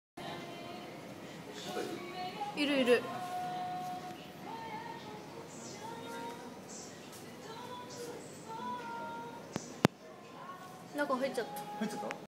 某ファーストフード店ネズミ
私が飼っているのは「カラージャービル(スナネズミ)」です
動画の撮影時に冷静なのは同じげっ歯類を飼ってるという理由もあります